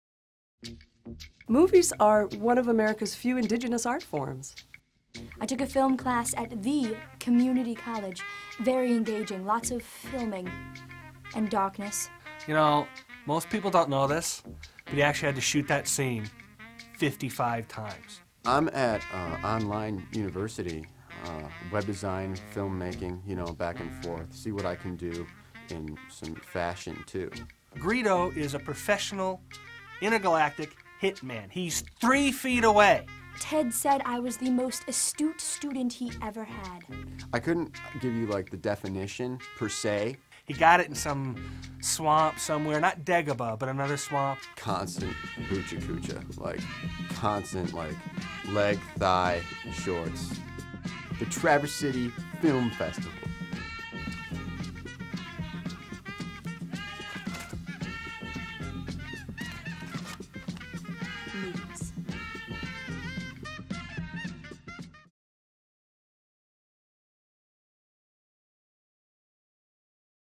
Traverse City Film Festival - Movie as an Art Form
One of the first bumpers from the original series of trailers produced for the inaugural Traverse City Film Festival in 2005. Unlike the 2006 effort, all these trailers were approved to play before the films.